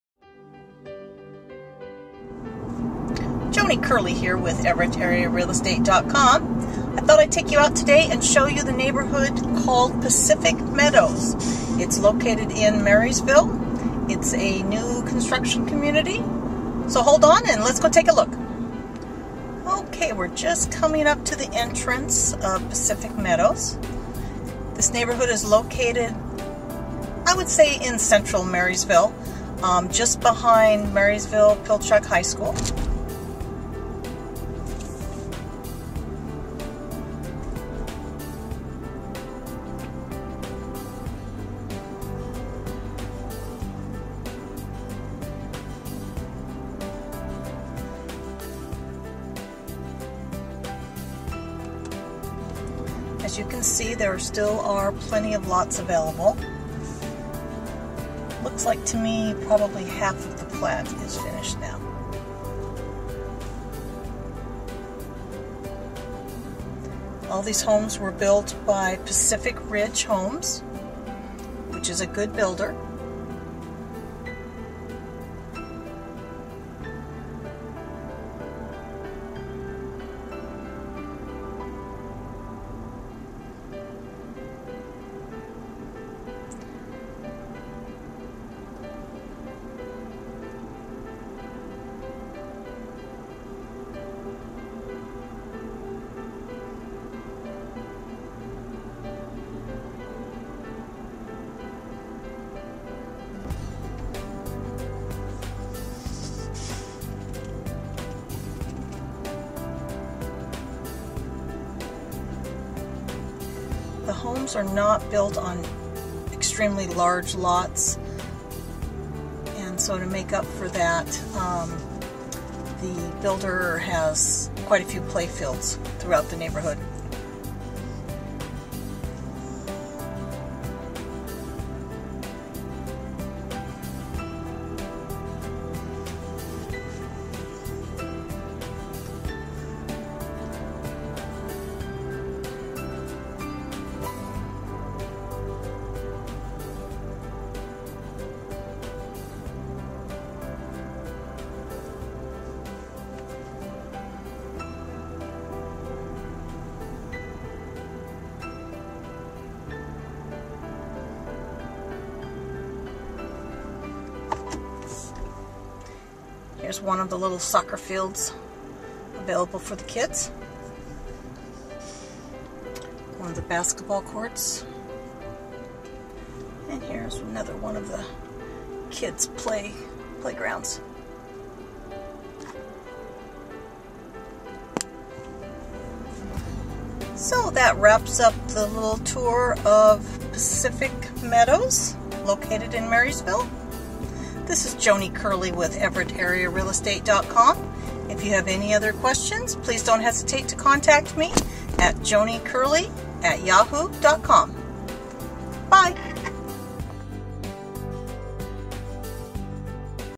Pacific Meadows Neighborhood Marysville WA
Video tour of the neighborhood "Pacific Meadows" in Marysville Wa. Pacific Meadows neighborhood is located in the central area of town and close to schools and shopping.  All the homes were built by Pacific Ridge Homes.

Joni Kerley - Broker
CENTURY21 Real Estate Center